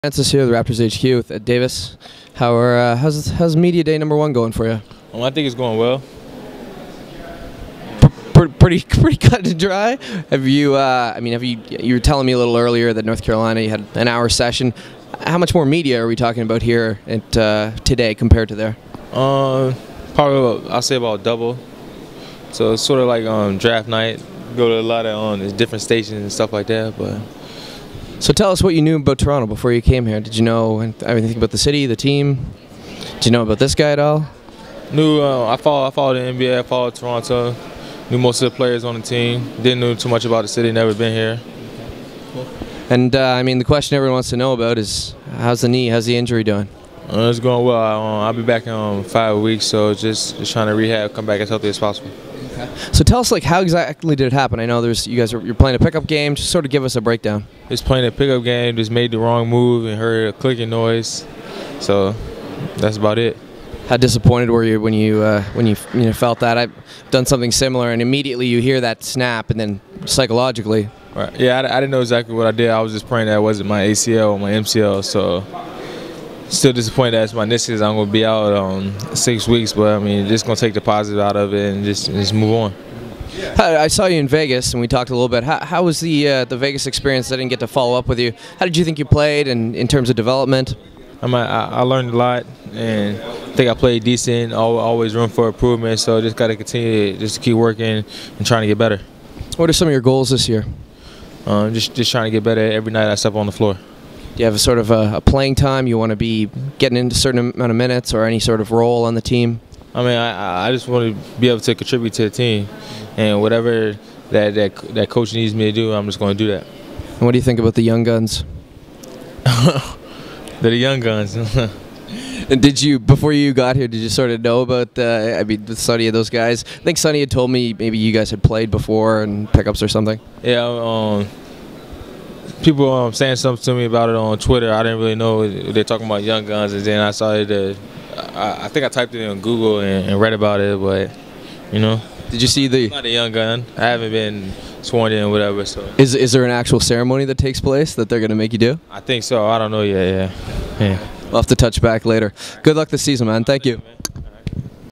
Raptors HQ.com - Raptors Media Day 2010 - Ed Davis Interview 720P
Adam Francis of Raptors HQ talks with Ed Davis about the upcoming season.  Davis talks about his first media day and how things compare to college. With special guest Jarrett Jack (blink and you'll miss him!)

Shot with a Canon T2i on September 27, 2010  - Downscaled to 720P